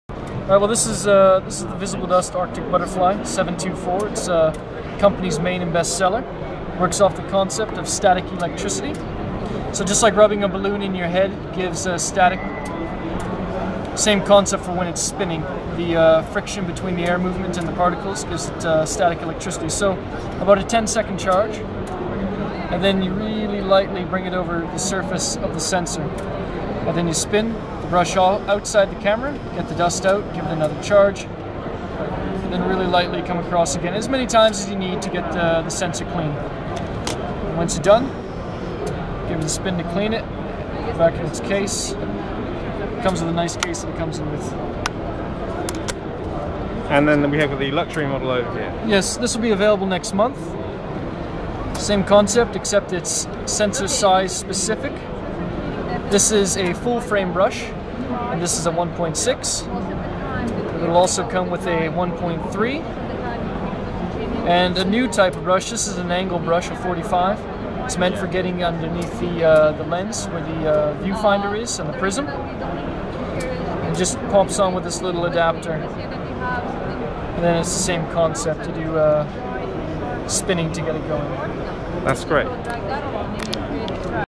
Arctic Butterfly DSLR camera sensor cleaner
A system special brushes for cleaning the sensor in your digital SLR camera. The brushes are spun by an electric motor in the handle to charge them with static electricity prior to use. This video was shot at the Focus on Imaging Show, Birmingham, UK, in February 2007, using a Nokia N93 camera phone. If trials of this camera prove successful, DPNow.com will use it for future show coverage and interviews.